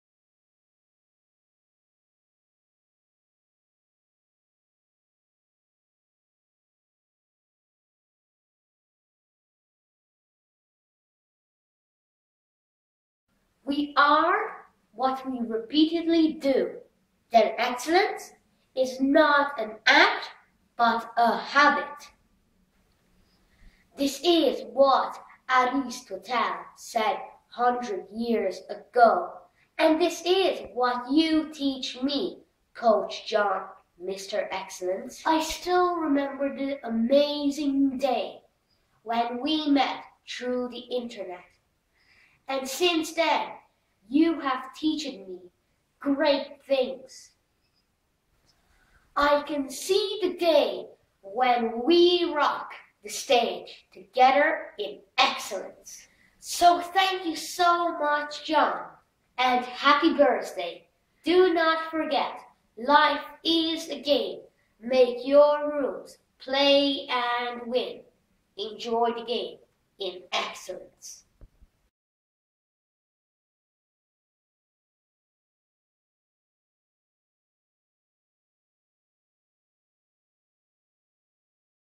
HAPPY BIRTHDAY TO THE BEST COACH: JOHN RAMSEY MR EXCELLENCE!
Happy Birthday John! Thank you for all the moments and all the quidance that you gave and will give to me! With Love and Respect Eric, The Game Changer!